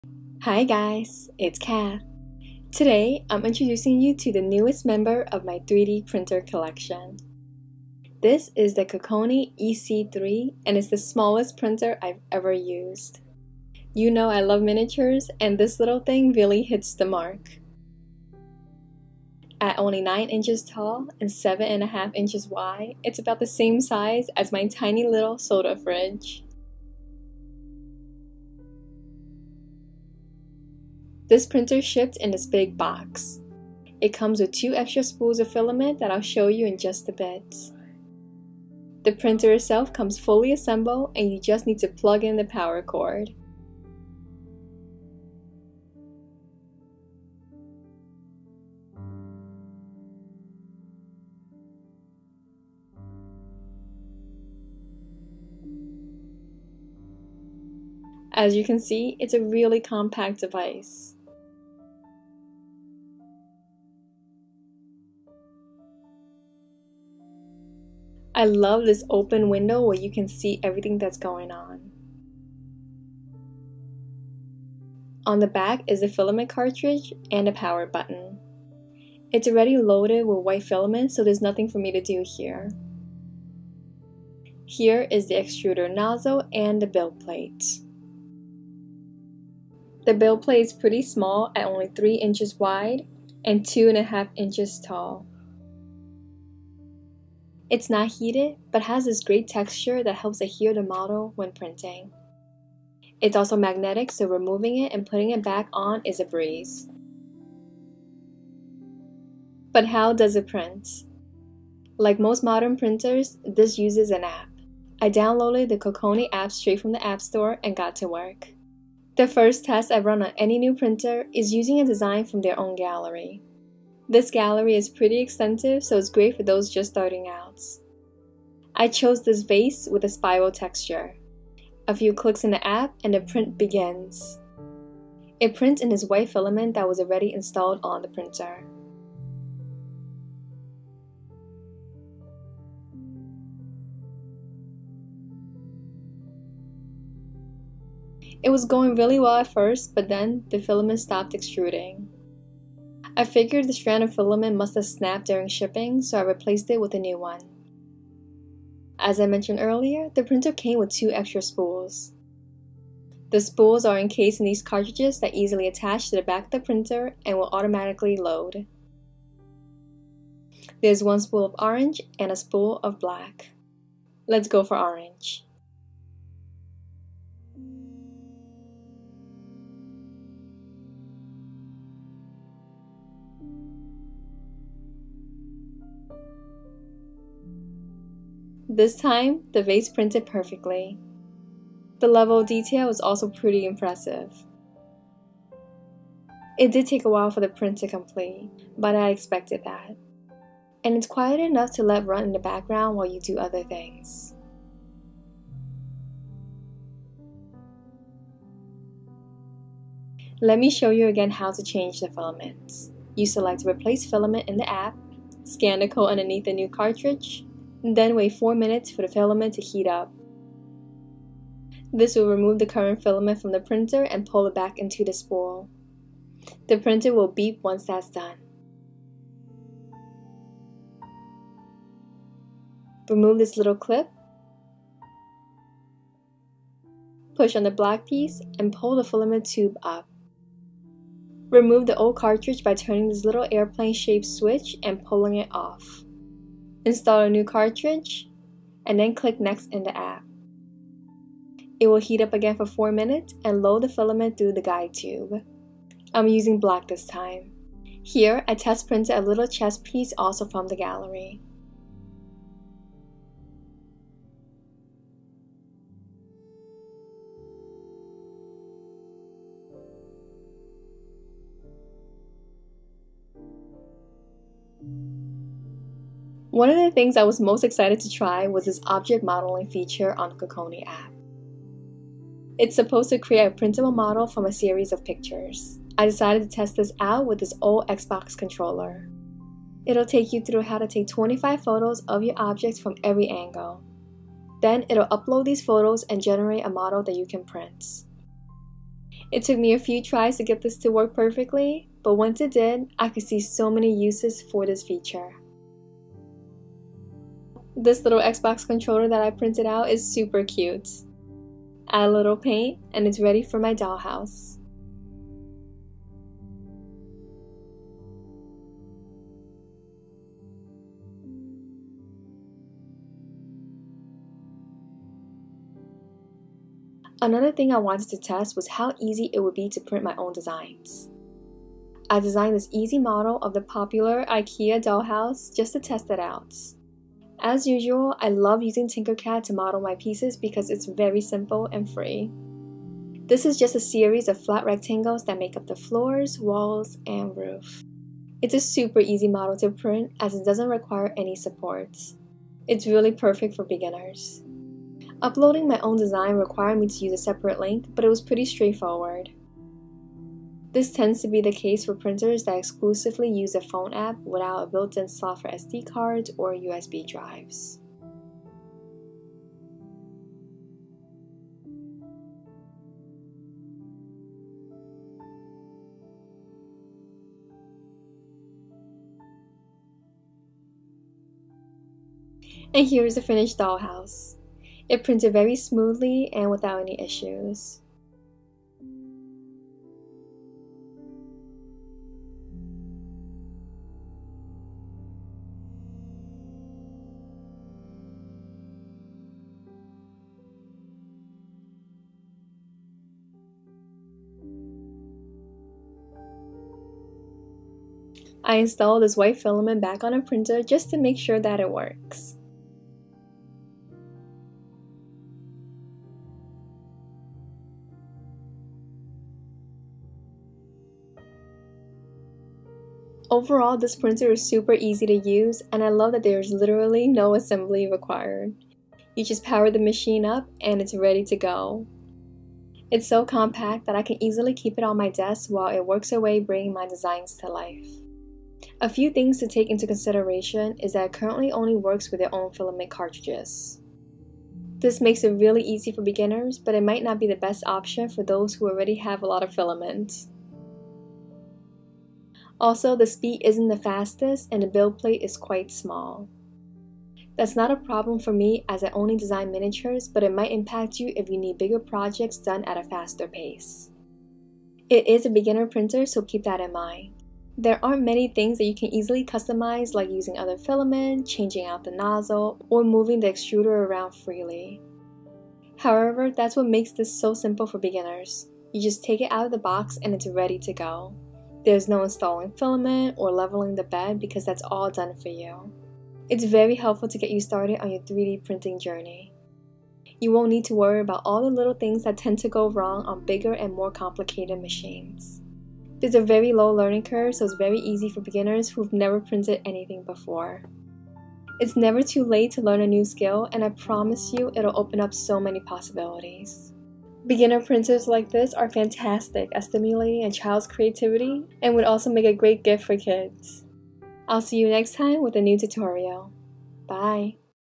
Smallest 3D Printer for Dollhouse Miniatures
Hi guys! Today I'm introducing you to the newest member of my 3d printer collection: the Kokoni EC1. It's a super small beginner printer that I'm testing out. How well does it print and is it worth purchasing? I go through all the details in this video. I hope you find this information useful!

$219.99